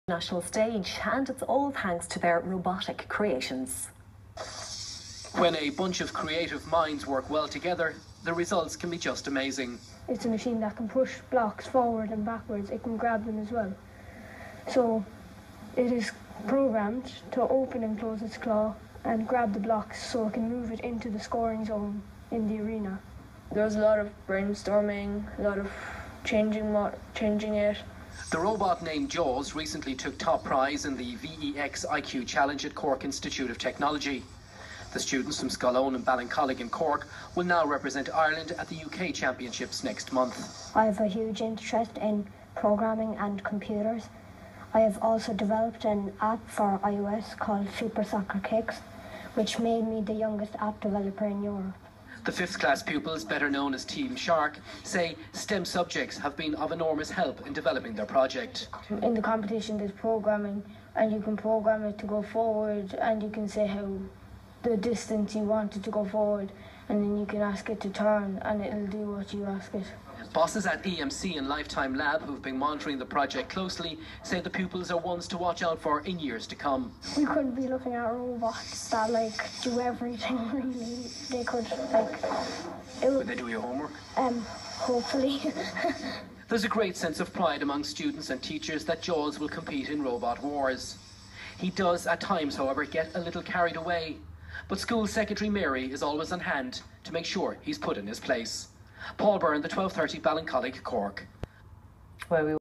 Scoil Eoin TV3 interview-VexIQ competitions
Scoil Eoin won all Ireland "Vex IQ" primary schools robotic competition and now they are going to UK finals in Birmingham.